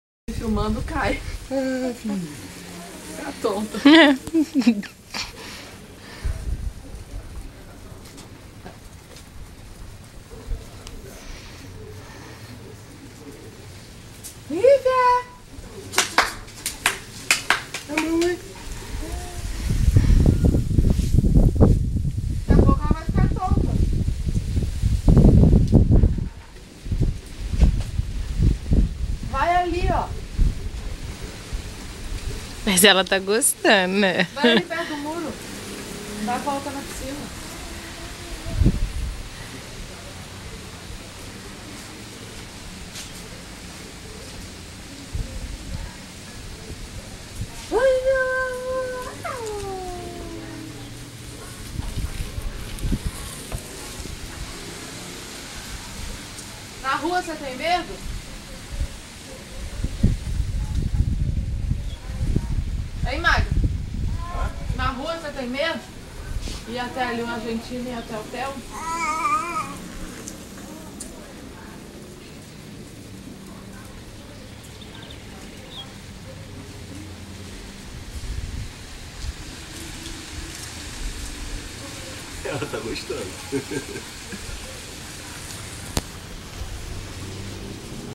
Livinha passeando com o papai na bicicleta da mamãe.
FELICIDADE!